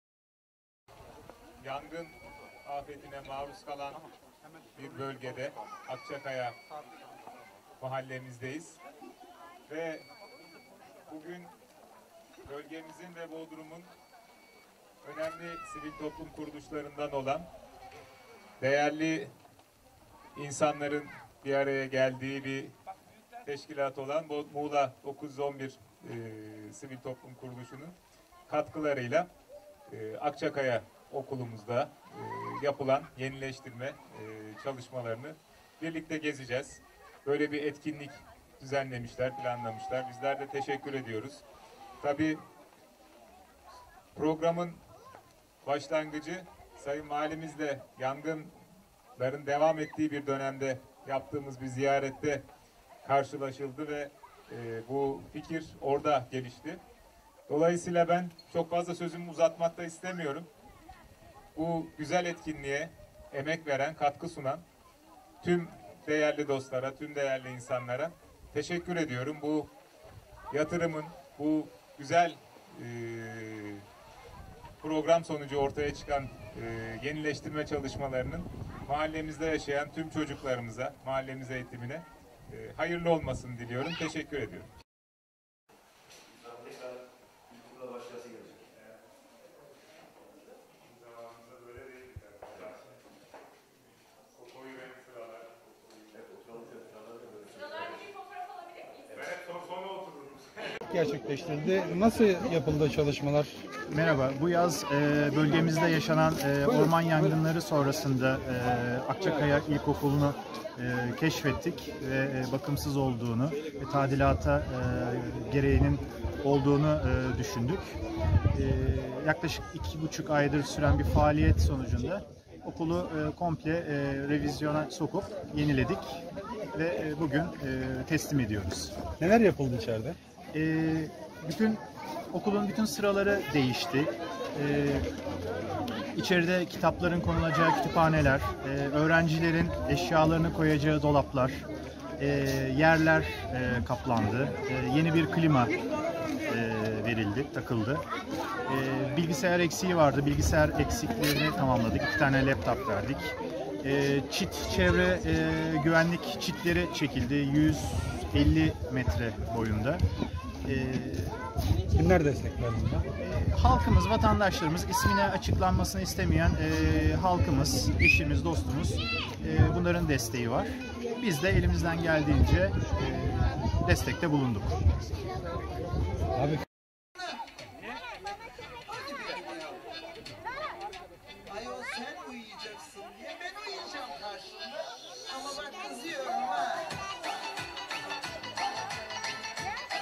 Muğla 911 Arama Kurtarma Araştırma Derneği Okulun Onarımına Destek Verdi
Detay İçin Tıkla